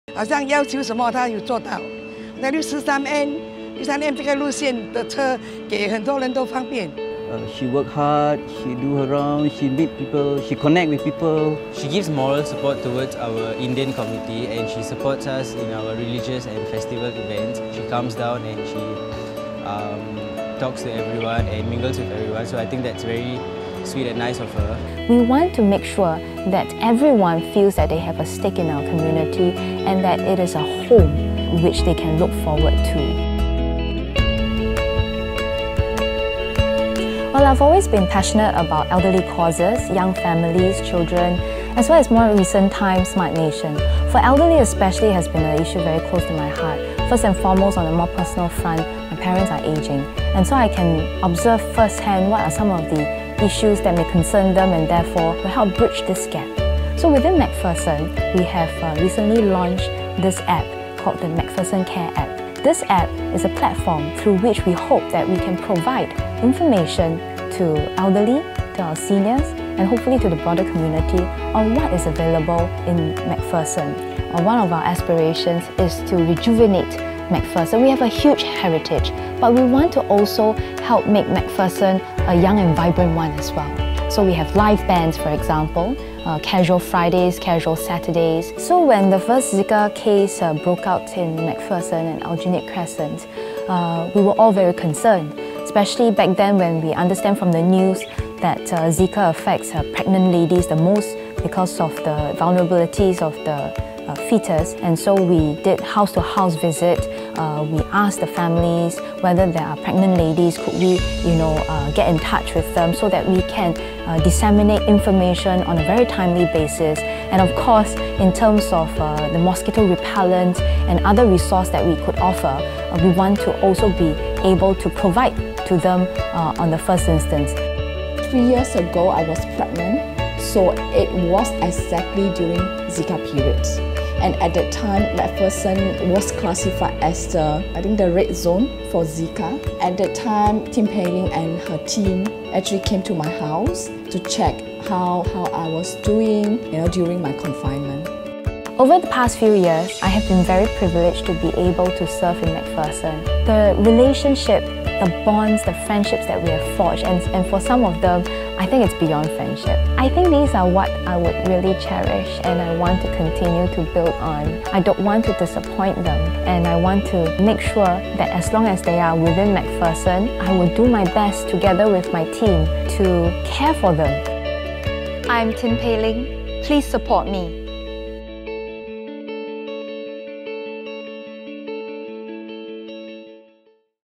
PAP Candidate: Tin Pei Ling
PAP Candidate for MacPherson SMC Tin Pei Ling: "Over the past few years, I have been very privileged to be able to serve in MacPherson. The relationships, the bonds, the friendships that we have forged and for some of them, I think it's beyond friendship. I think these are what I will really cherish and I want to continue to build on. I don't want to disappoint them and I want to make sure as long as they are within MacPherson, I will do my best, together with my team to care for them."